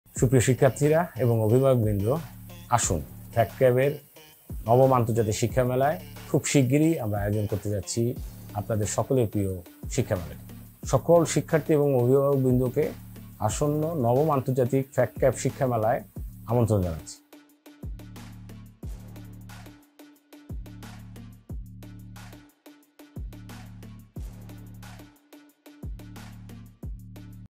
First Step to | Student Visa Processing | FACD CAB | 9th International Education Expo 2024
The Honorable General Secretary of FACD CAB welcomes you all to the largest education expo in Bangladesh.

🎓Know more about lucrative university offers in your desired country.
𝐅𝐀𝐂𝐃 𝐂𝐀𝐁 𝟗𝐭𝐡 𝐈𝐧𝐭𝐞𝐫𝐧𝐚𝐭𝐢𝐨𝐧𝐚𝐥 𝐄𝐝𝐮𝐜𝐚𝐭𝐢𝐨𝐧 𝐄𝐱𝐩𝐨 𝟐𝟎𝟐𝟒.